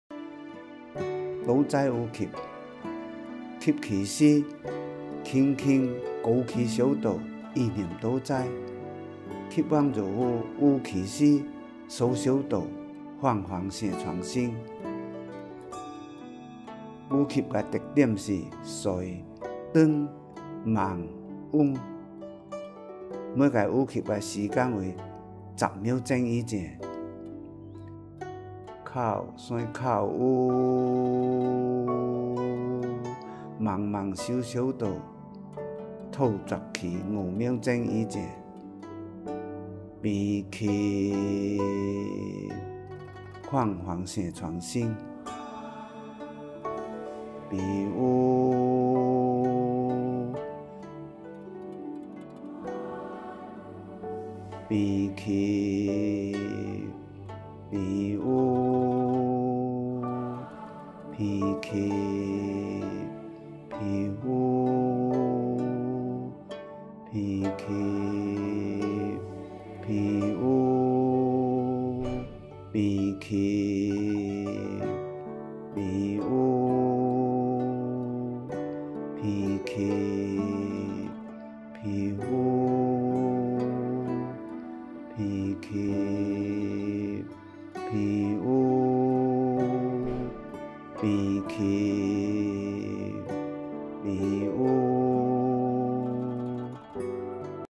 Centennial Project Practice for Health: Relaxation and Brain-nourishing Method 8/11百岁工程养生法放松养脑法8/11
85 years old Traditional Chinese Medicine Doctor Xiao Xinhe (Dr. Ming Wu's mentor. He is a retired professor from Guangzhou University of TCM) demonstrating the Relaxation and Brain-nourishing Method